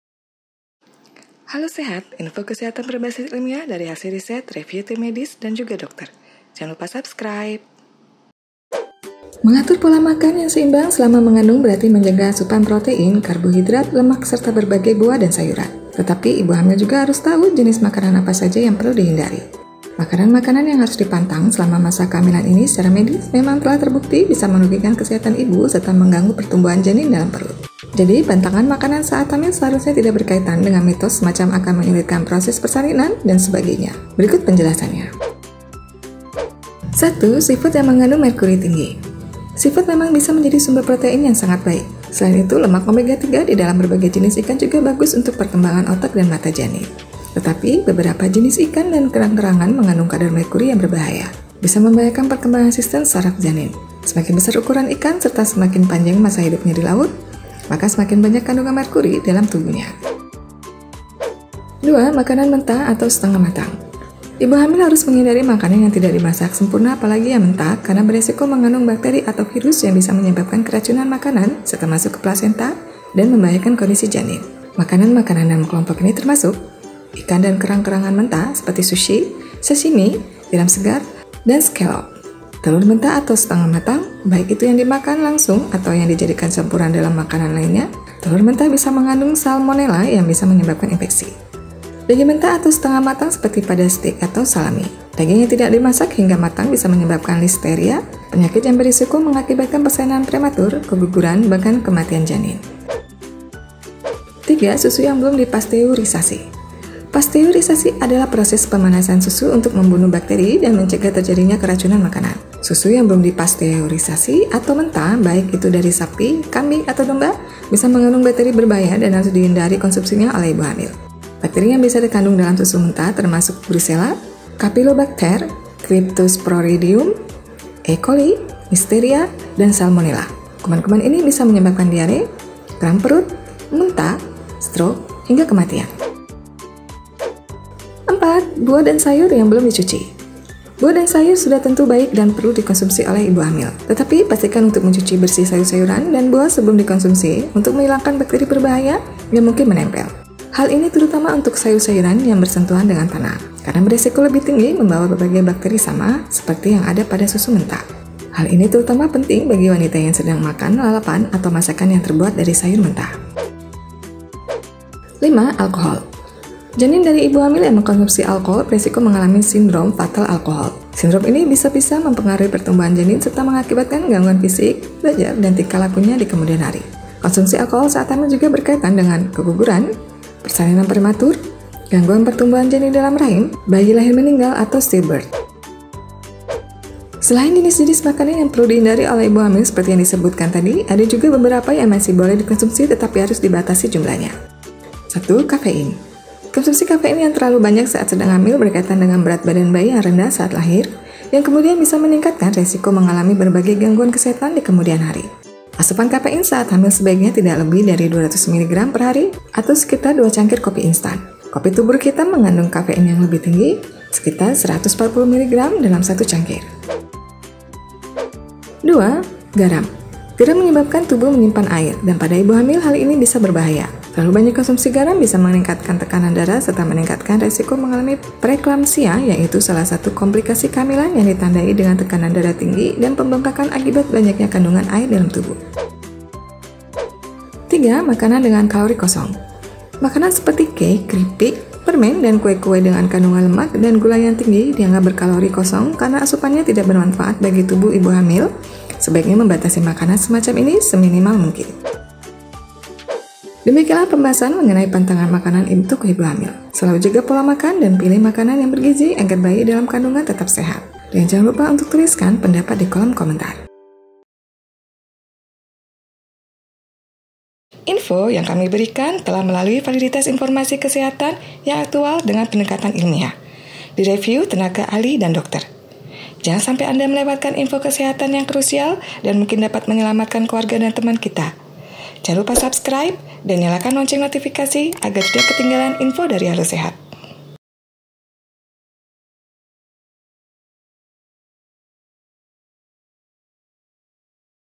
Pantangan Makanan Saat Hamil yang Wajib Dihindari
Apa saja makanan yang wajib dihindari oleh ibu hamil?

Medical Research & Source:
1. Cleveland Clinic Team. What Not to Eat When You’re Pregnant. Cleveland Clinic Health Essentials; 2020.
2. Tim Newman, Natalie Butler, R.D., L.D. Which foods to eat and avoid during pregnancy. Medical News Today; 2017.
3. Mayo Clinic Staff. Pregnancy nutrition: Foods to avoid during pregnancy. Mayo Clinic; 2021.
4. British Nutrition Foundation. What not to eat when pregnant. Nutrition.org; 2021.